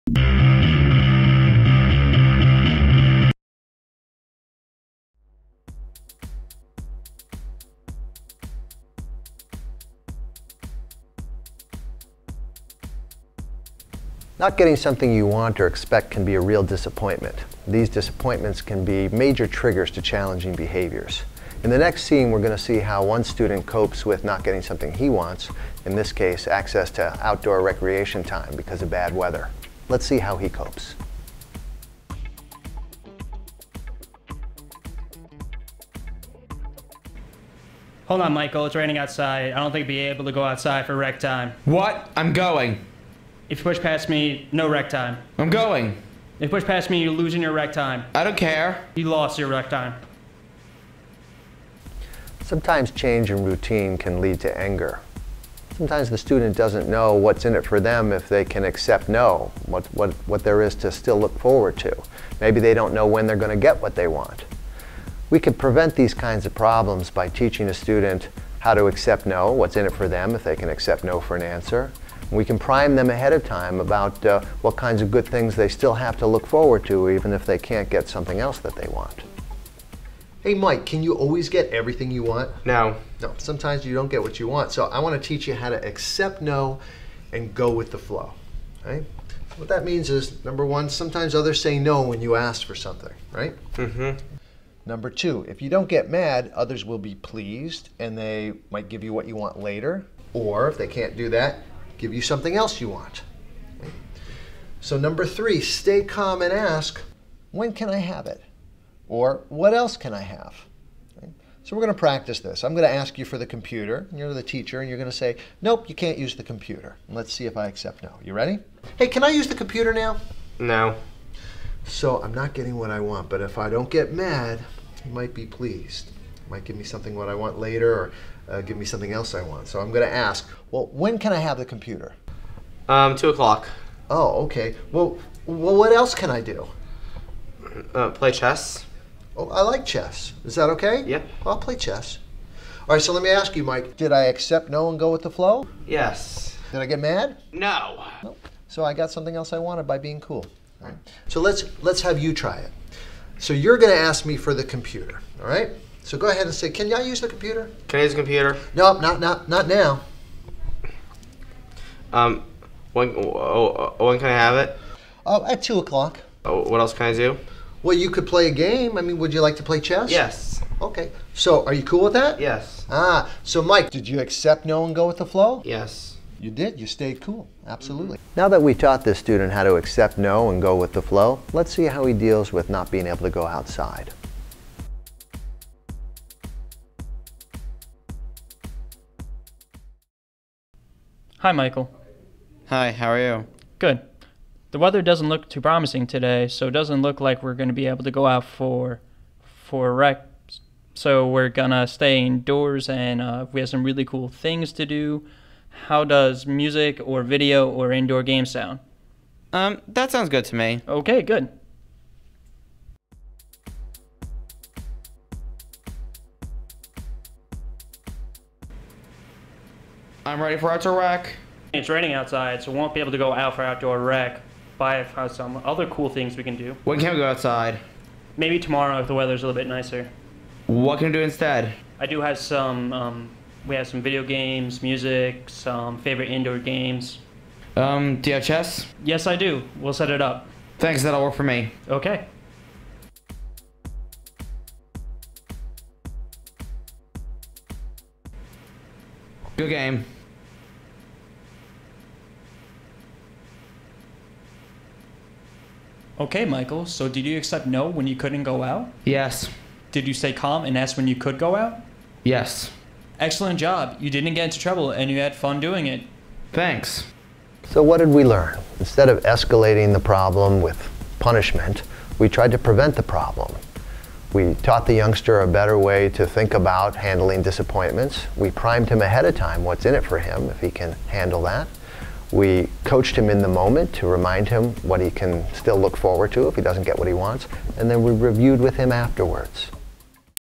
Accepting Change At School
Learning to accept no when one cannot have something desired. In this video, the student learns what is in it for him if he can deal with the disappointment of not being able to go outside for recreation time. The skills modeled is that there is always something else to live for if one cannot have something. Students are taught to ask, " when can i have what i want?" and "what else can i have or do?" These questions help the student stay aware of positive things that will happen rather than focus on what they cannot have.